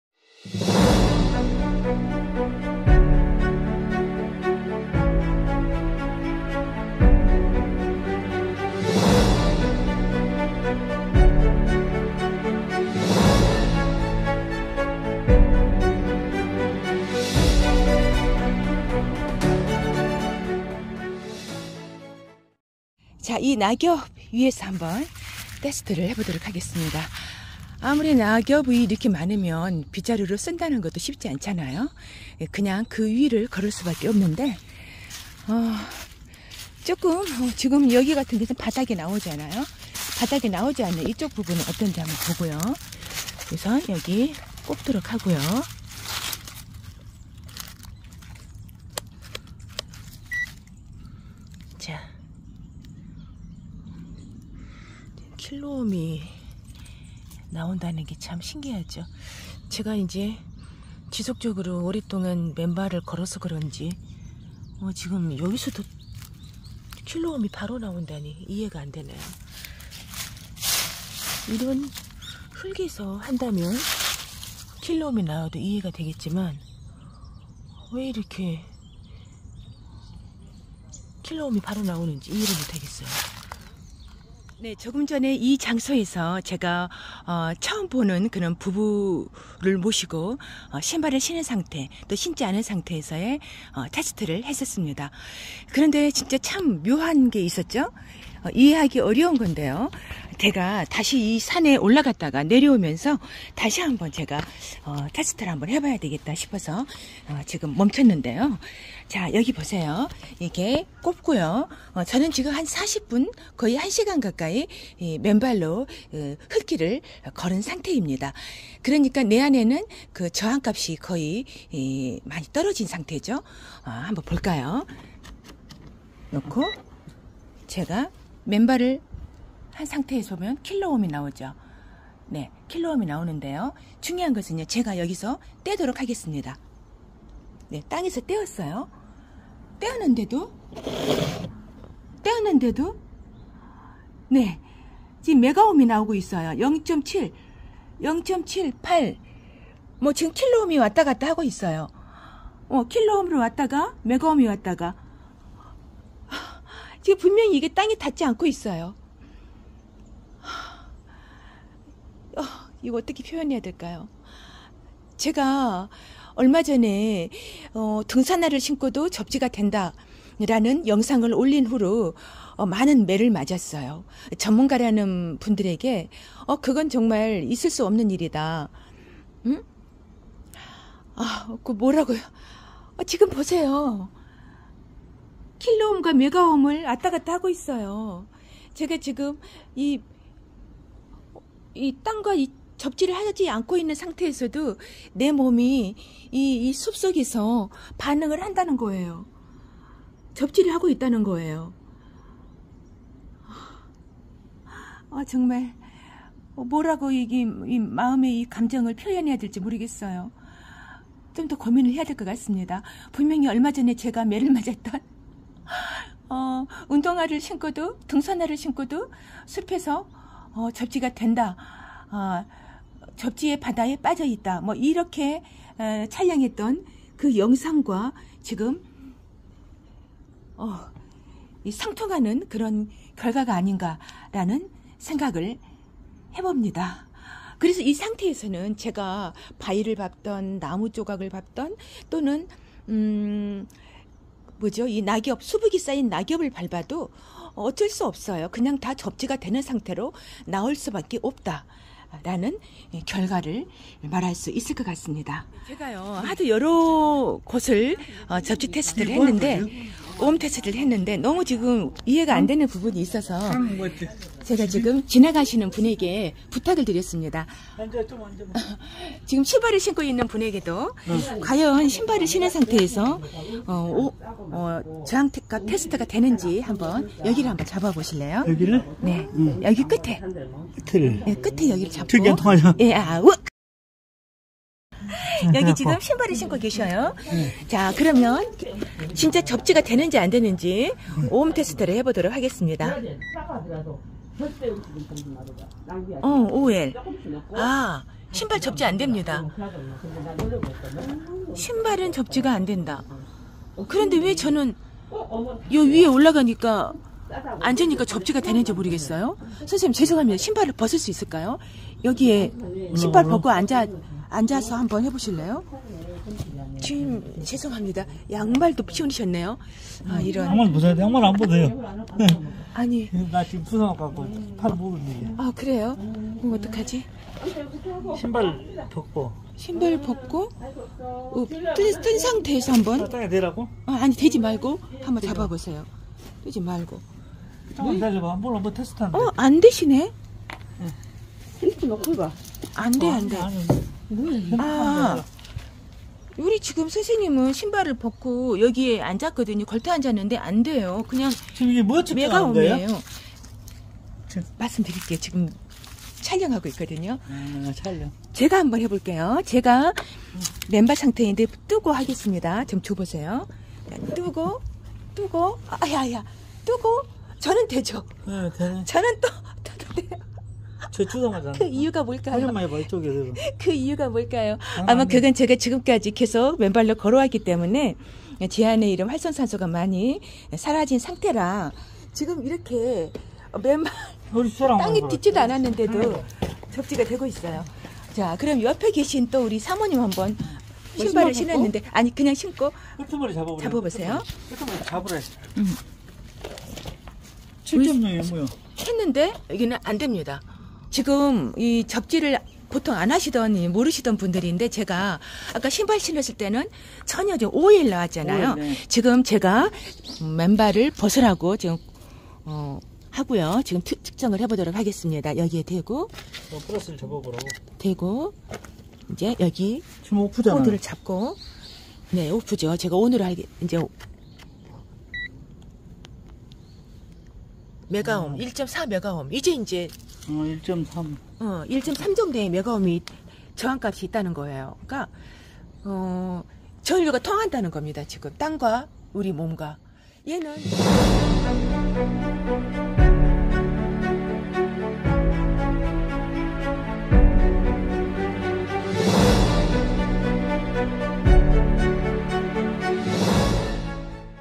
[맨발걷기53] 흙에서 발떼고도 접지가 되는 마법같은 현상/ 누가 비밀을 풀 수 있을까요
다음 영상을 참조하십시오
이 영상과 연관성이 있는거 같습니다.